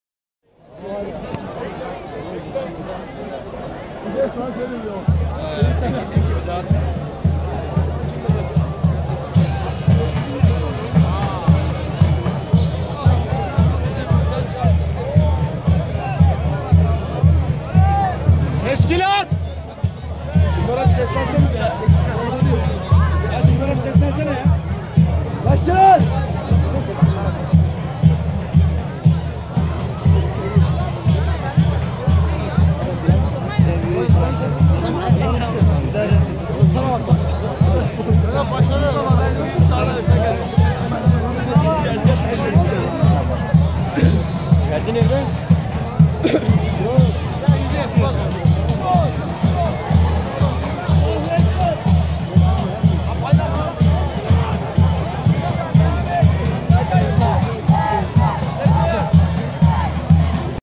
102. Yılında Sarıkamış : Kalabalık Artıyor!
Sarıkamış Harekatının 102. yıldönümü nedeniyle yapılacak olan etkinlikler için Sarıkamış'a hareket ettik. Güvenlik kontrolünden sonra yürüyüş alınına geçtik ve herkesin gelmesini beklemeye başladık. Alana binlerce kişi gelmişti. Kalabalık sürekli olarak artıyordu. Artık hareket saati neredeyse gelmişti.